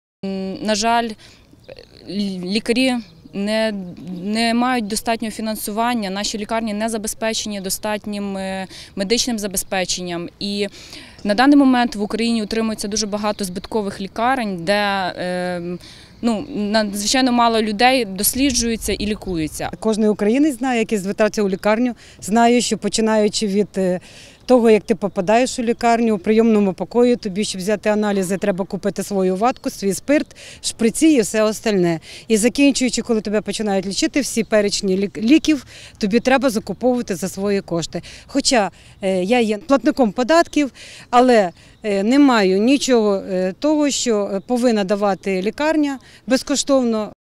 У Хмельницькому відбулася «Година смерті»
«Година смерті» на підтримку медреформи. У Хмельницькому активісти влаштували під стінами Будинку Рад мовчазну акцію. Так представники пацієнтських організацій та активісти закликАли  депутатів підтримати медичну реформу в Україні. Окрім плакатів із закликами учасники Всеукраїнської акції намалювали на асфальті силуети тих, хто помер від неякісної медицини. Їх лише за годину в Україні нараховують майже 70. Учасники акції переконані: змінити ситуацію може лише реформа.